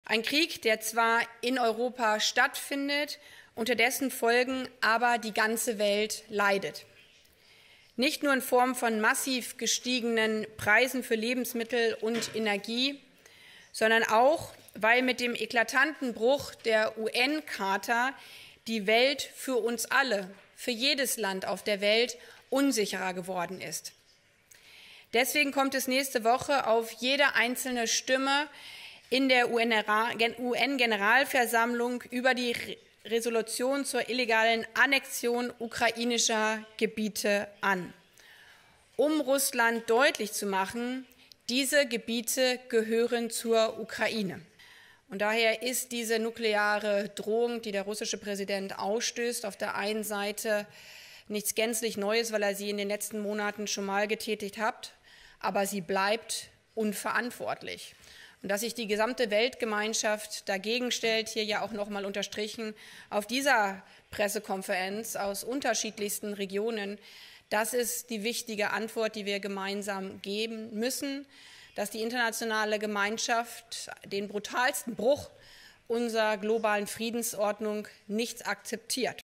Baerbock: "Die Gebiete gehören zur Ukraine"
Nächste Woche wird die UN-Generalversammlung über eine Resolution zur Annexion ukrainischer Gebiete abstimmen. Bundesaußenministerin Annalena Baerbock fordert ein geschlossenes UN-Votum gegen Russland.

Bildrechte Thumbnail: John MacDougall/AFP/GettyImages